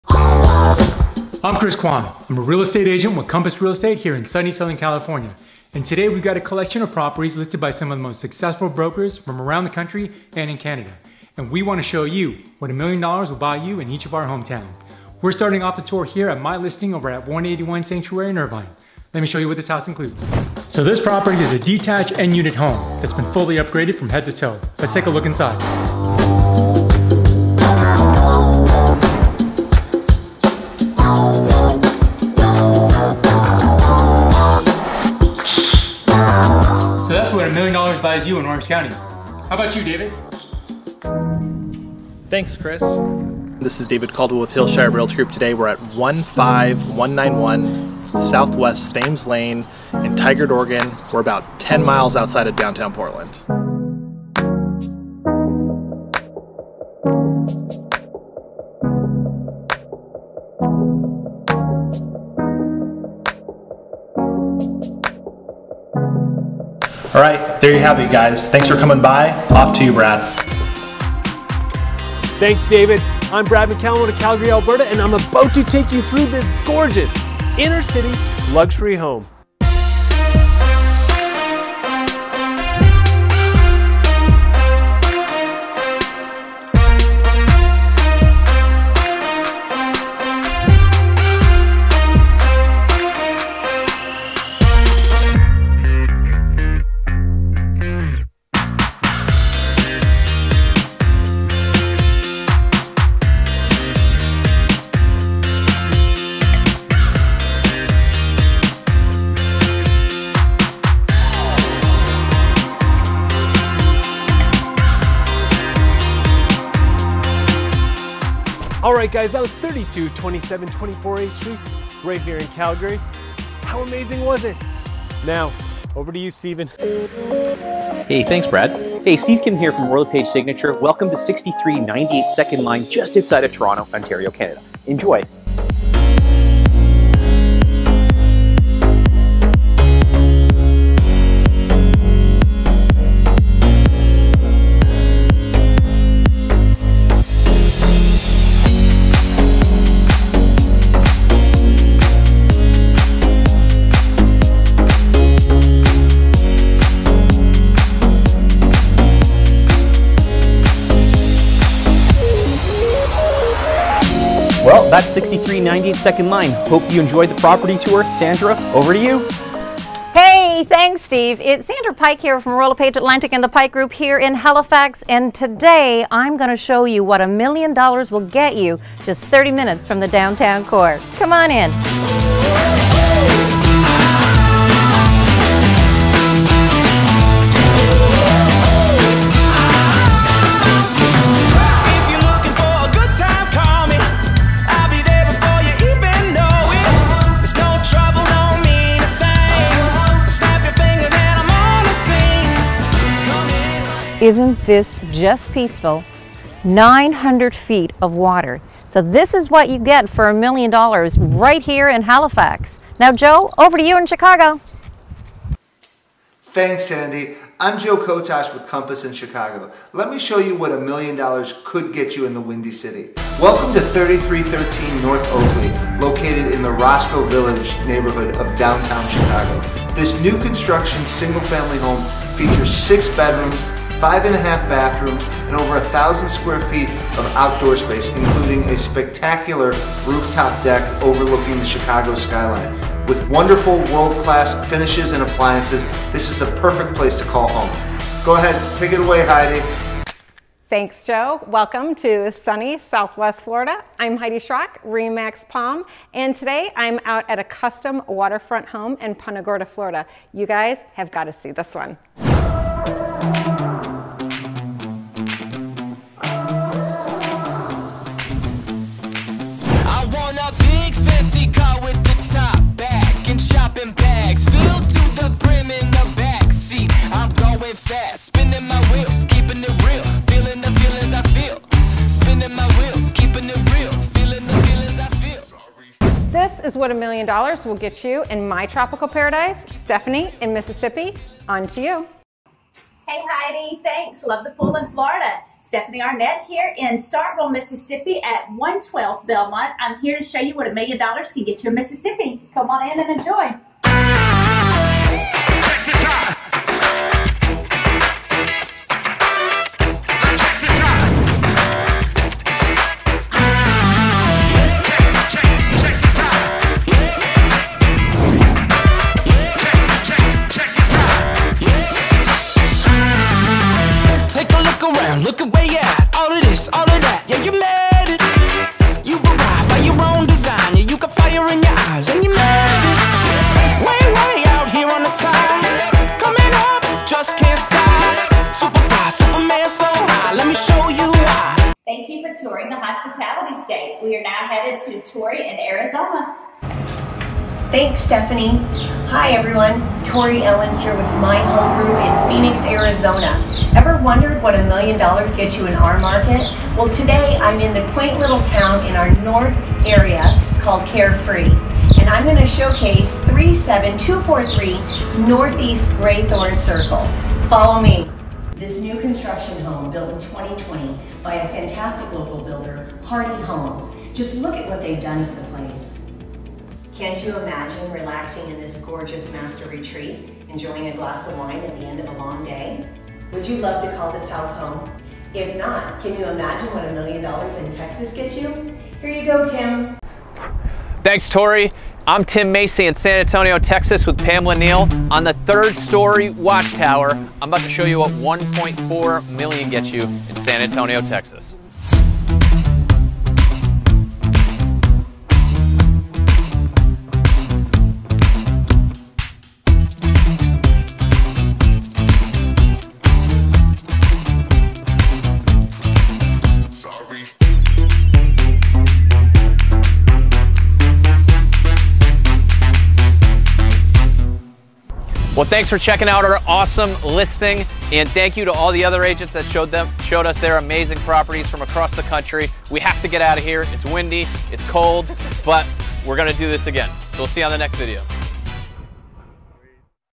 Million Dollar Project Series
Ever wonder what a MILLION dollar home looks like in Florida? Texas? Calgary? Toronto?

I sure did. For some reason, I thought that the homes would be smaller. Less blingy. BOY, I was WRONG.

Sit back and for the next 7 minutes take this million dollar tour.

A big shout out to all of the agents who participated, you guys rock. Scott Davis our producer was so kind to edit this for us. You are amazing. Many thanks.